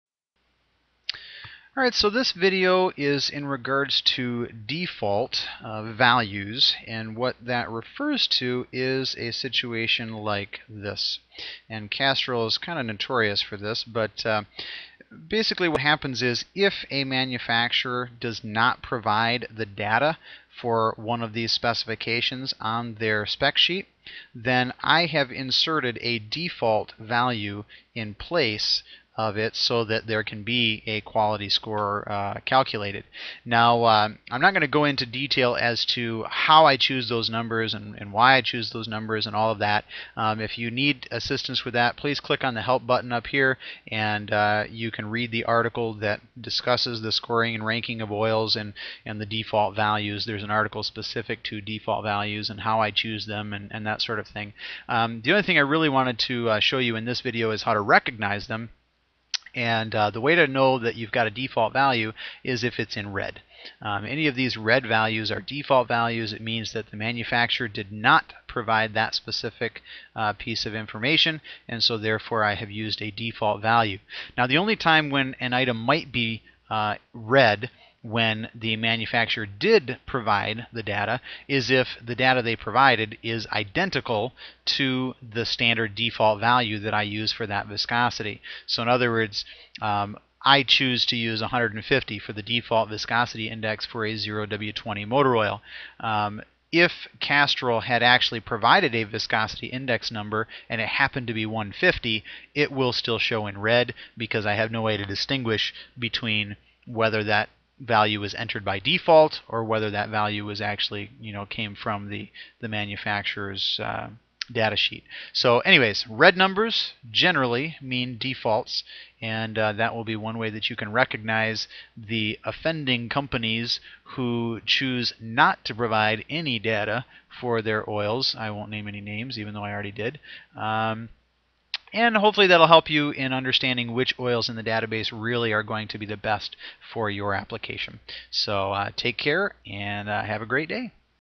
The Motor Oil Evaluator Online Oil Comparison Application
- Database of more than 1000 petroleum and synthetic motor oils.  Interactive online application allows you to sort and filter this list quickly and efficiently to find the BEST oil for your application.